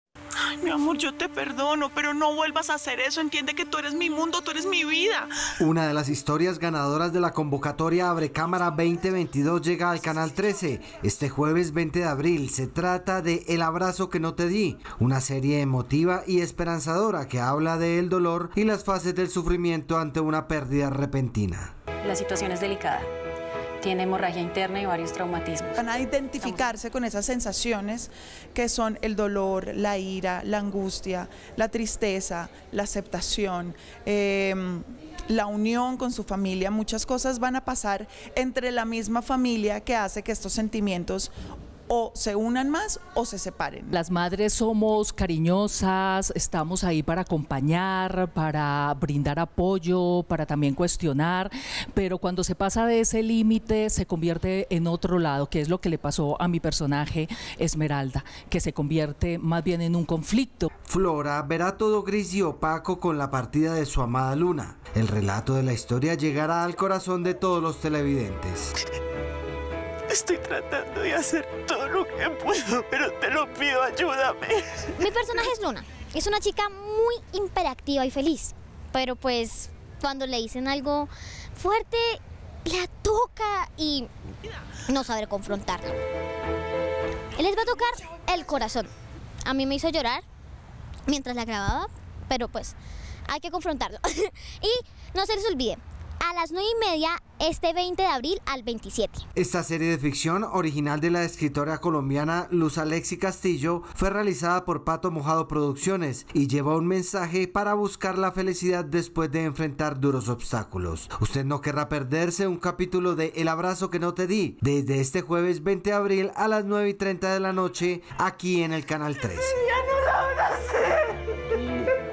'El abrazo que no te di', estreno en Canal Trece | Noticias Enlace Trece
🟠📺 EnlaceTrece | 📺🎬👨‍👩‍👧‍👦💕 Este jueves 20 de abril llega a nuestro canal ElAbrazoQueNoTeDi, la serie ganadora de la convocatoria Abre Cámara 2022. Una producción que nos invita a reflexionar sobre la importancia del amor de nuestros seres más cercanos